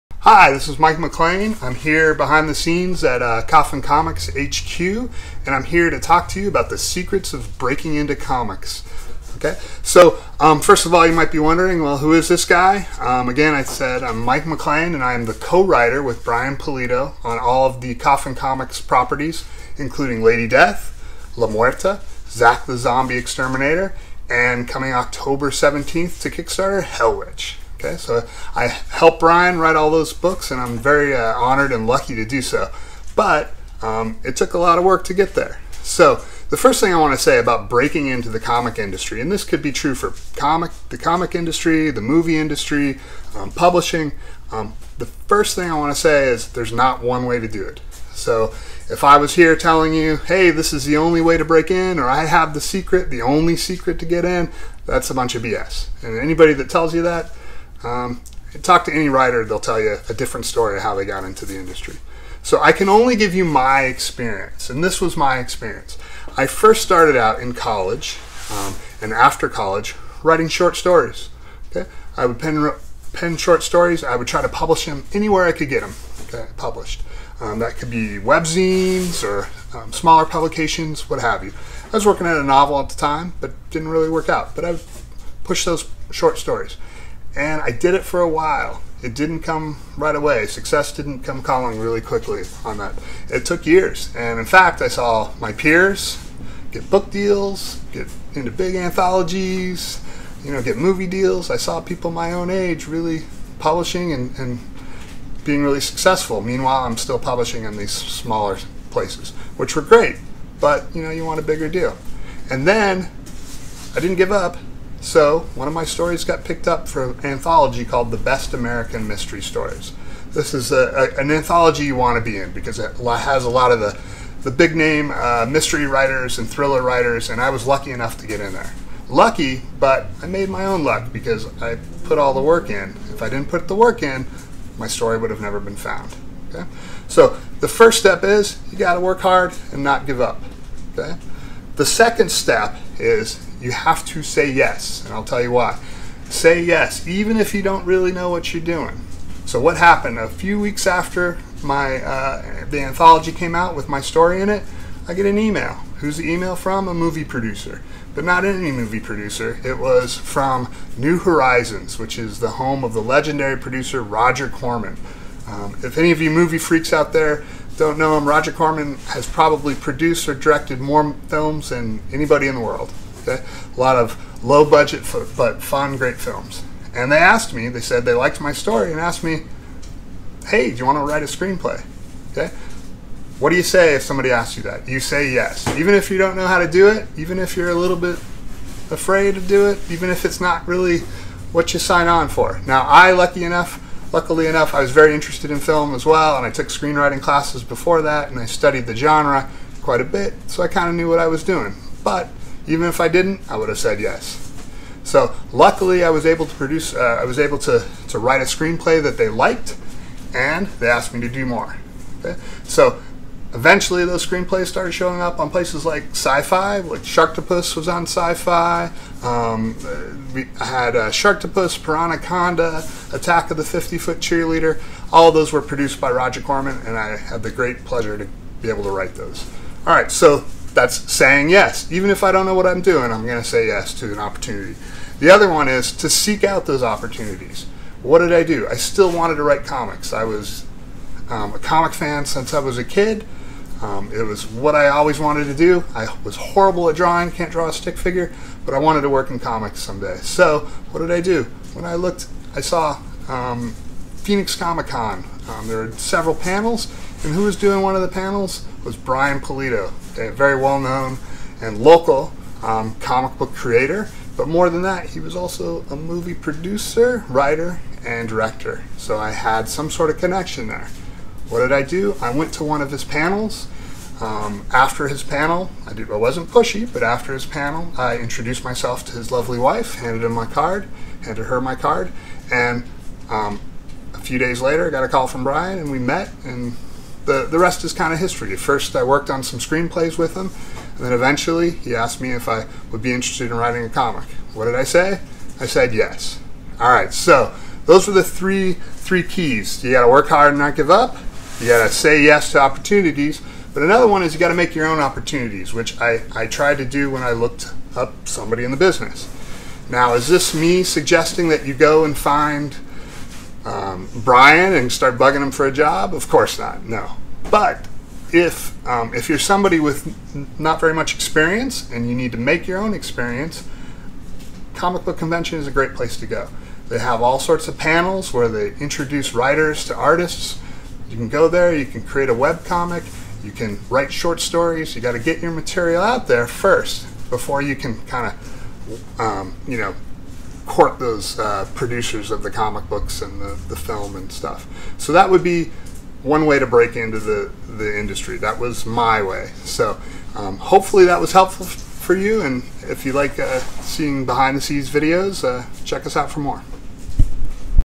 Episode 2. Comic Book Writing With Mike MacLean - 3 Keys To Breaking Into The Comic Book Industry
In this episode Mike MacLean talks about the 3 keys to breaking into the comic book industry.

GET CONNECTED!

FB Messenger: m.me/LadyDeathUniverse